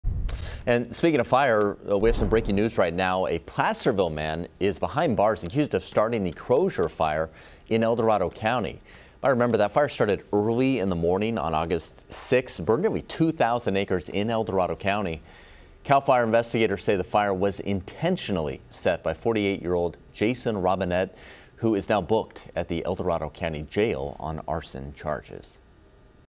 Placerville man accused of starting Crozier Fire
The fire started early in the morning on Aug. 6 and went on to bur nearly 2,000 acres.